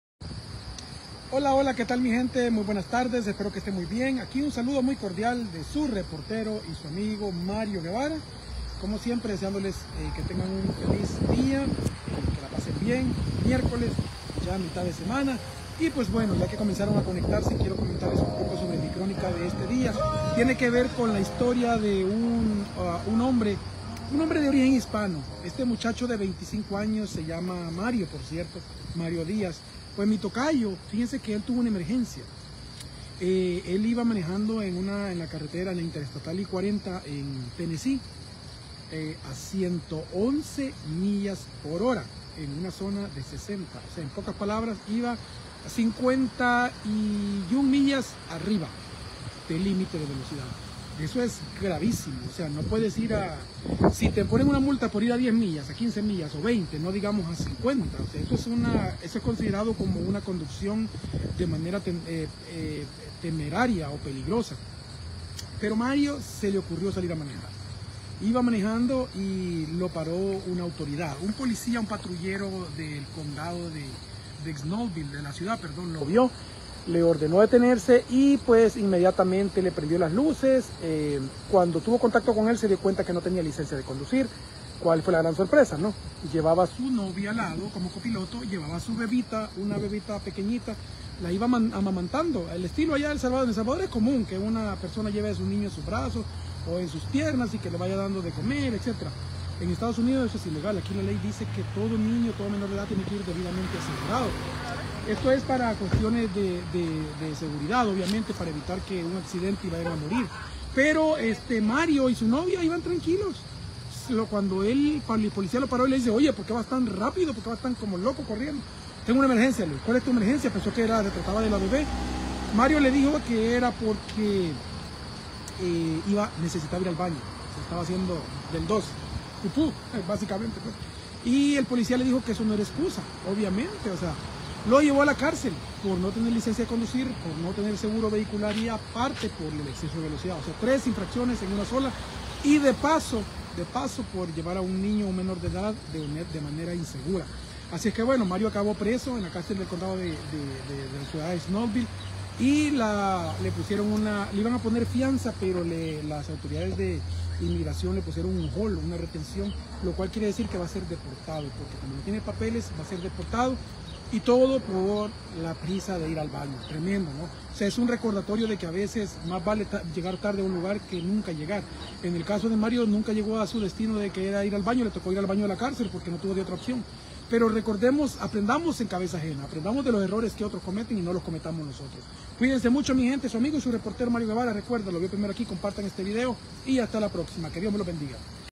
DEPORTADO Y TODO POR UNA NECESIDAD Mario Díaz quería hacer sus necesidades y terminó arrestado
DEPORTADO Y TODO POR SU URGENCIA DE IR AL BAÑO. Mario Díaz solo quería hacer sus necesidades y terminó cómo nunca imaginó ¿Le puede pasar a cualquiera?